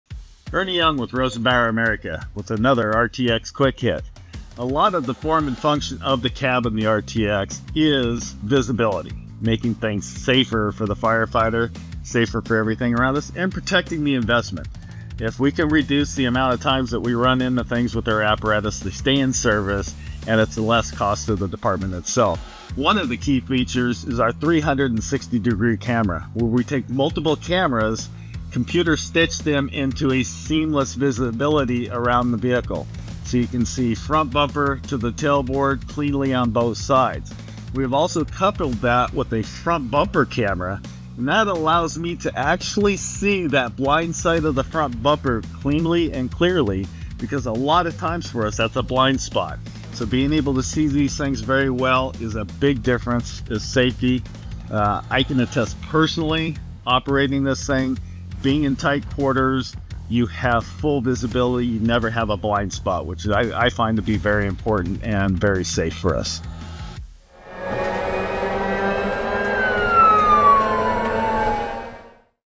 Rosenbauer RTX Cab Visibility
See the future of firefighting with crystal-clear vision in the Rosenbauer RTX electric fire truck! With a one-piece glass windshield, 360-camera system, night vision side mirrors, and front bumper camera, visibility has never been better. From navigating tight city streets to maneuvering through low-light conditions, the RTX ensures firefighters have an unparalleled view of their surroundings at all times.